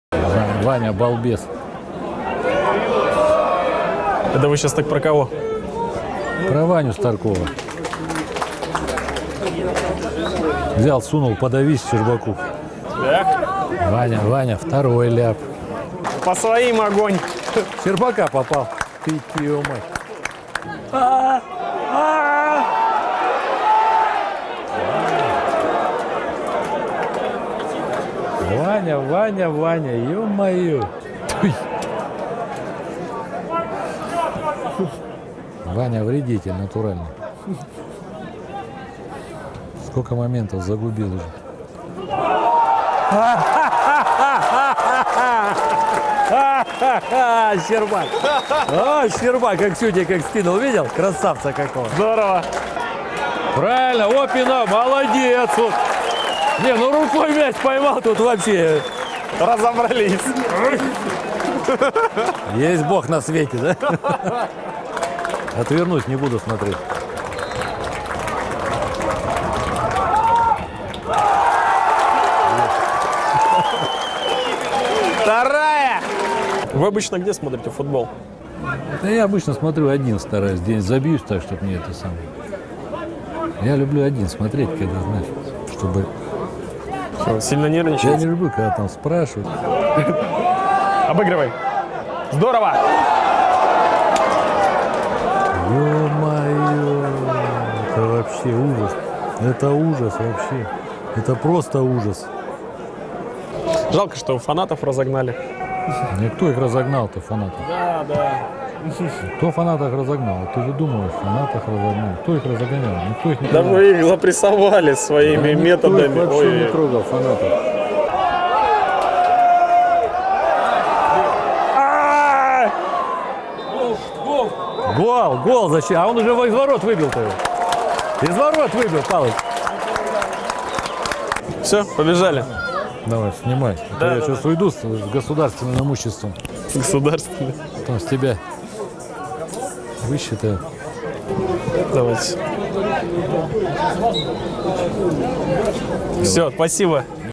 Матч от первого лица: как выглядит алтайский футбол из вип-ложи?
Порадовал поклонников футбольный клуб «Динамо». «Сине-белые» буквально уничтожили дублеров «Томи» — 4:0. Корреспондент Кирилл Политов отравился в «вип-ложу» и посмотрел матч вместе с генеральным директором клуба Виктором Сигаревым.